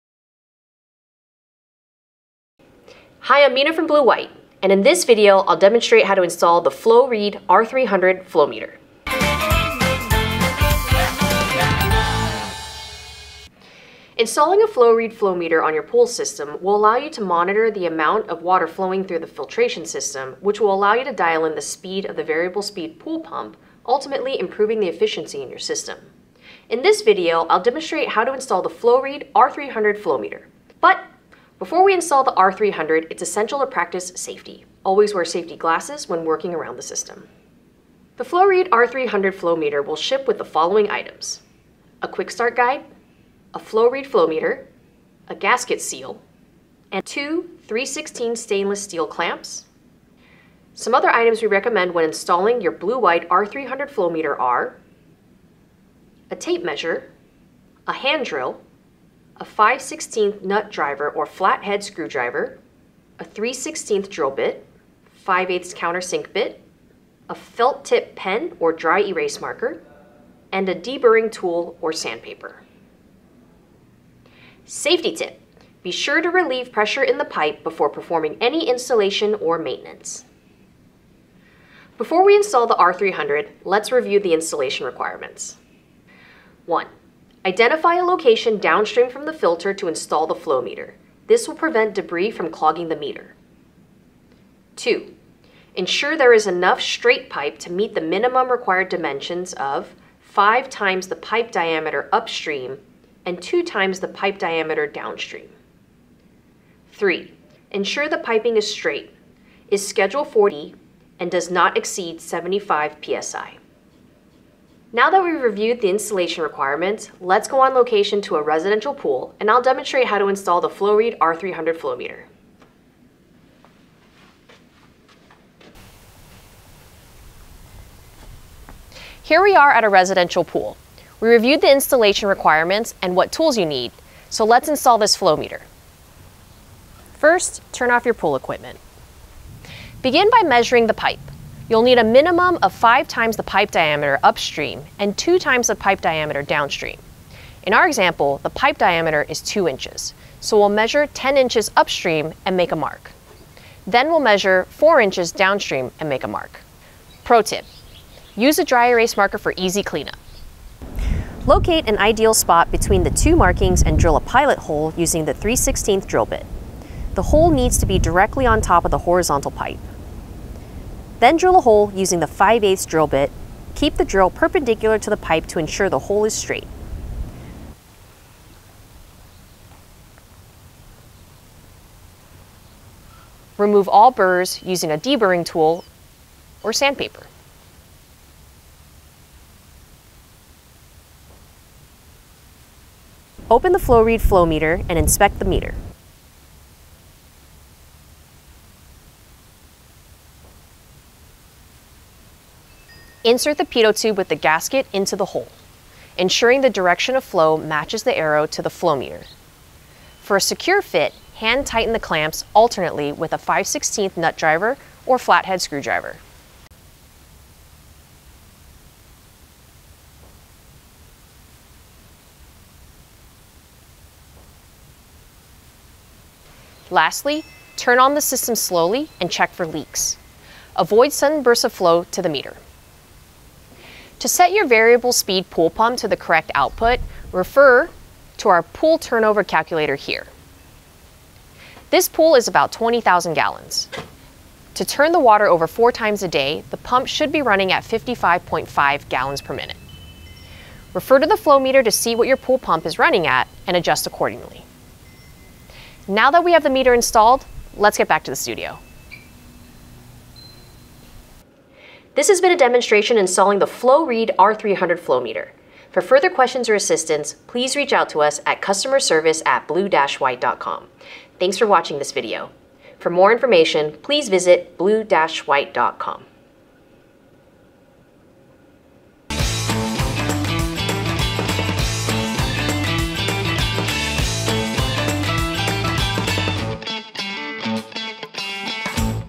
How To Install The R-300 FLOWREAD® Flow Meter In A Residential Pool System
In this video, Mina explains the importance of installing the FLOWREAD® R-300 flow meter in your pool system.

Mina goes on to explain what ships with the R-300 flow meter and what additional items we recommend for a successful installation.

After reviewing the installation requirements, Mina goes on-site to a residential pool to demonstrate how to install the FLOWREAD® R-300 flow meter in a pool system.

*Contents for the video:*
00:00 - Welcome!
00:14 - What does the FLOWREAD® R-300 do?
00:38 - What does it ship with?
00:50 - What additional items are recommended?
01:22 - What are the installation requirements?
01:55 - Installation on a pool system
03:53 - Using our pool flow calculator
04:21 - Questions?